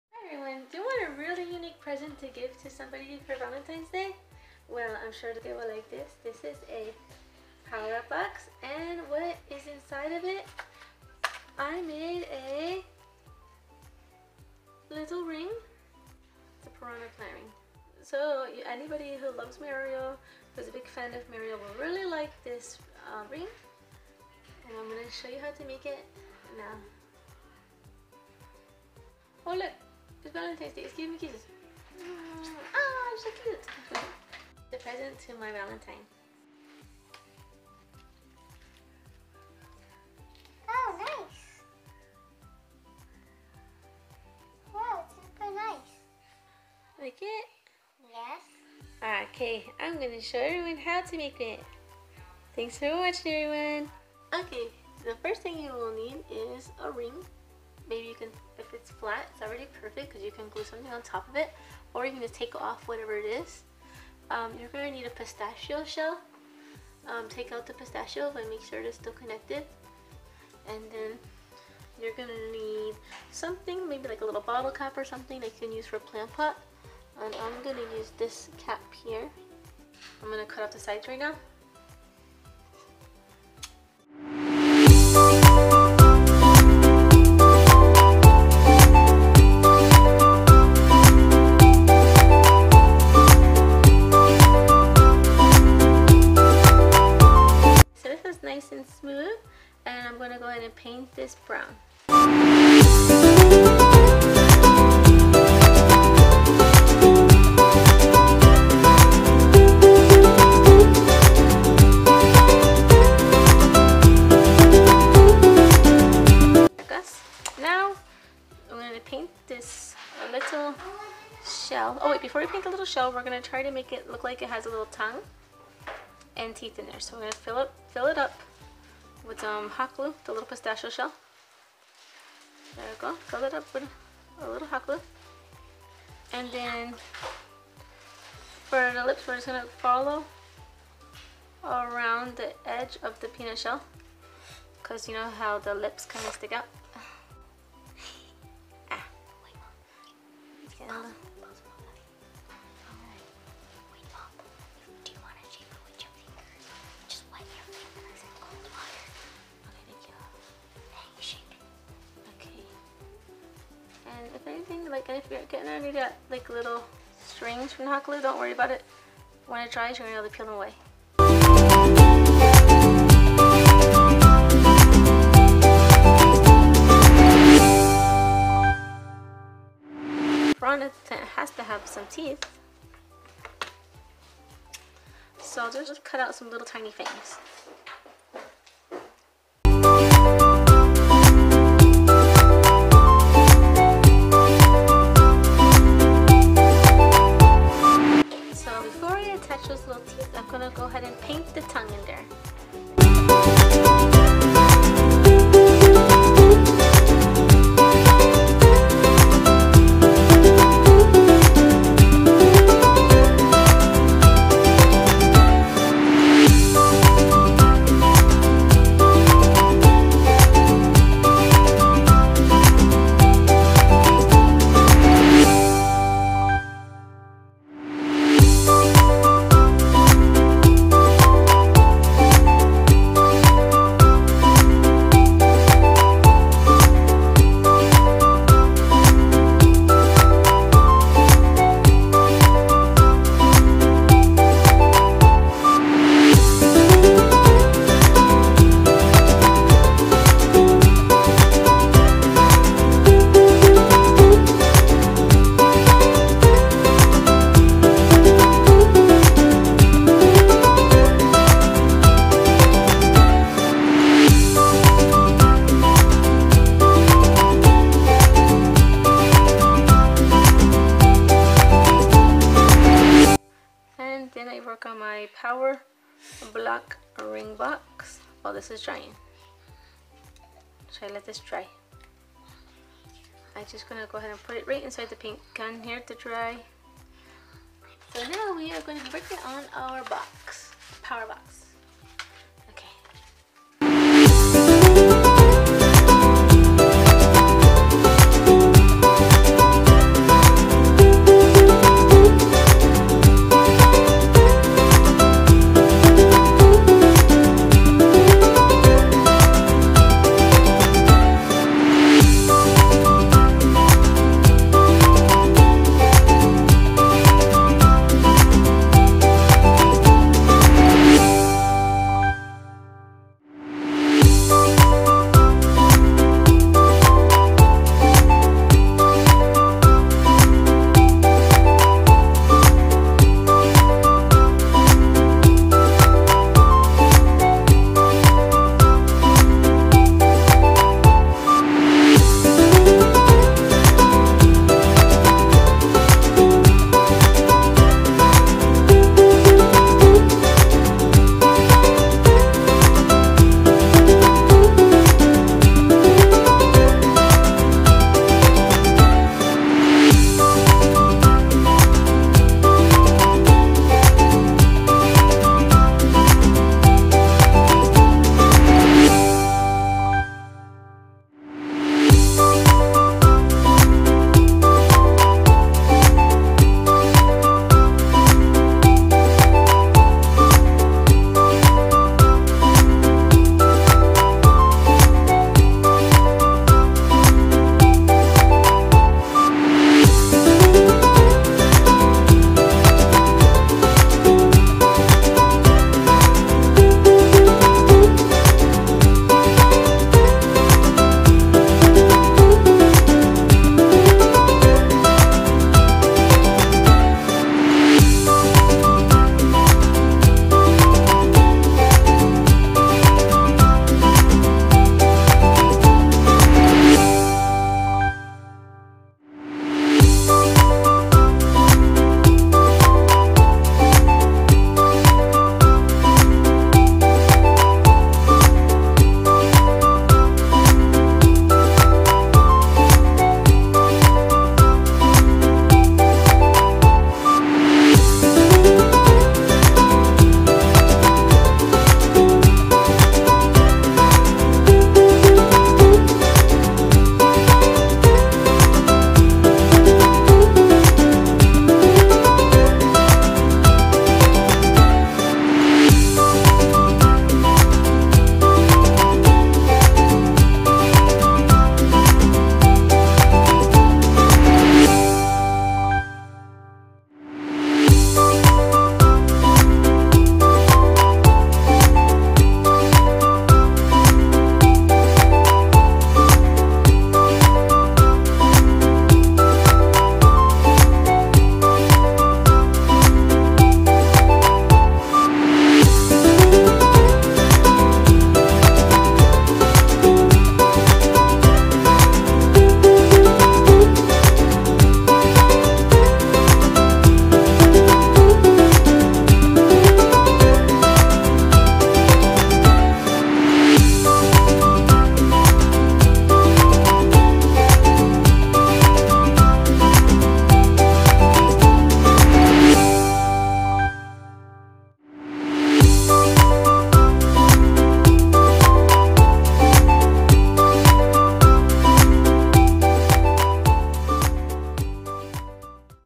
Valentines Piranha Plant Ring DIY
Your Valentine will love this cute piranha plant ring. It is made of really simple items from around your house.